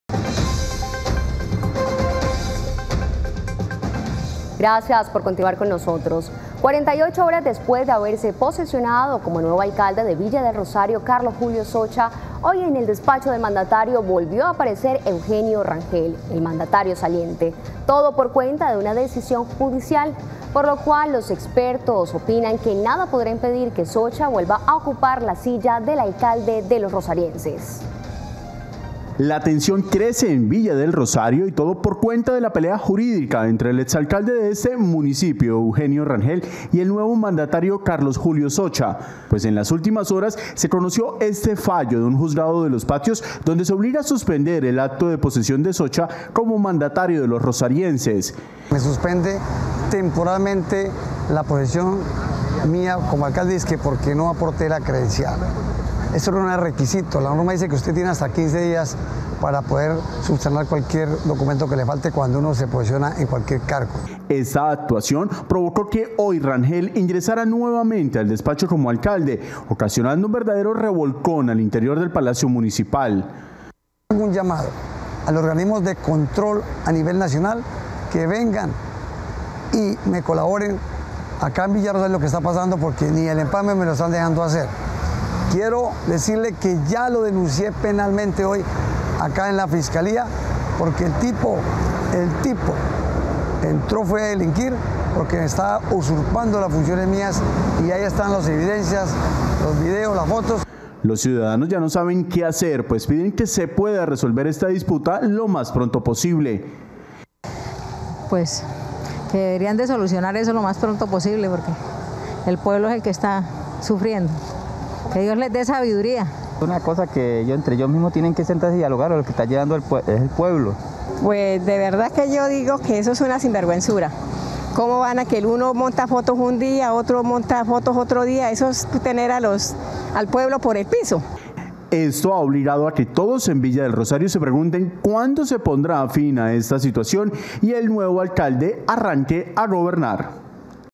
¿Quién es el alcalde de Villa del Rosario, Socha o Rangel?
Una confusión viven los habitantes de Villa del Rosario, Norte de Santander, en donde no hay un alcalde legalmente posesionado, debido a que, luego de dos días de haber tomado el cargo, a Carlos Julio Socha no le permitieron mantenerse en el puesto.